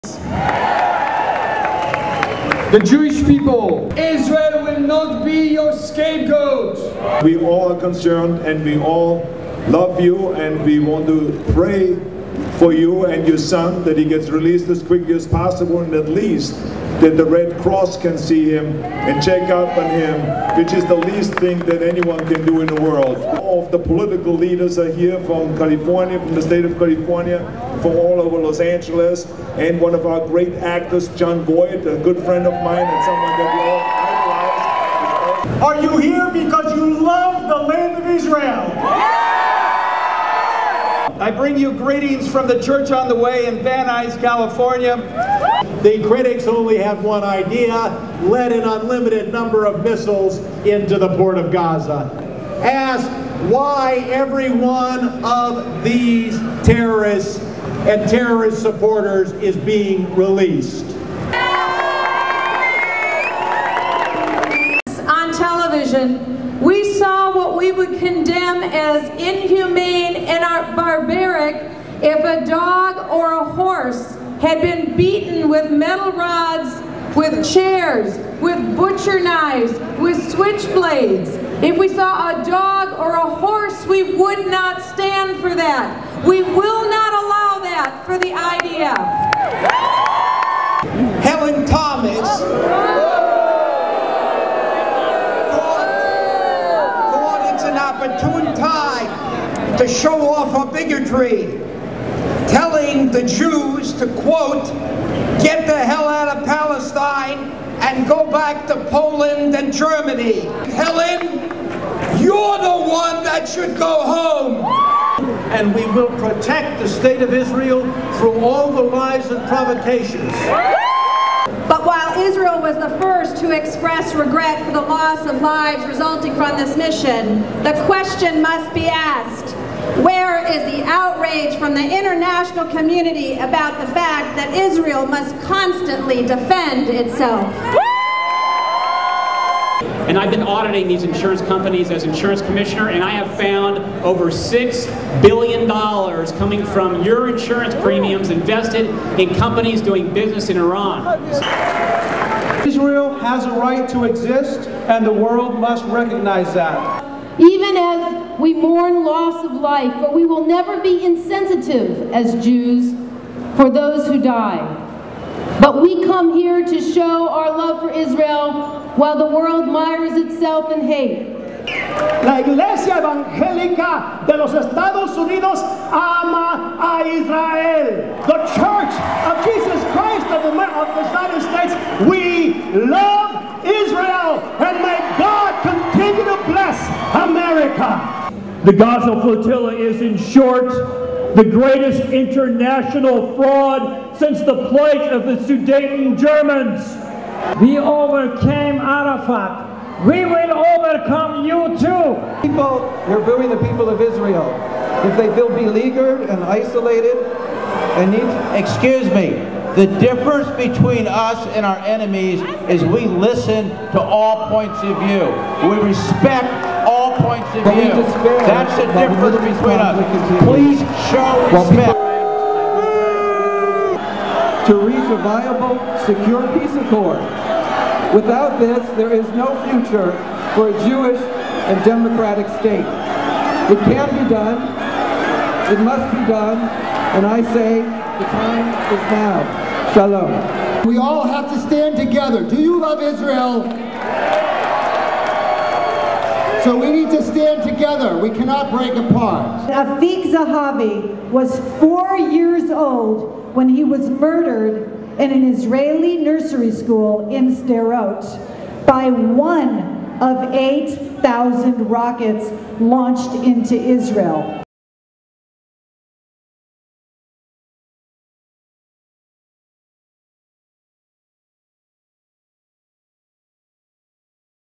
"Pro-Israel" Rally June 6 on Wilshire - HD version
Includes Governor Arnold Schwarzenegger, Rabbi David Wolpe, Rabbi Denise Eger, Representative Michele Bachmann, Pastor Jim Tolle, Representative Brad Sherman, Rabbi Marvin Hier, Jon Voight, Wendy Greuel, David Pine, Steve Poizner, Jay Sanderson, Roz Rothstein..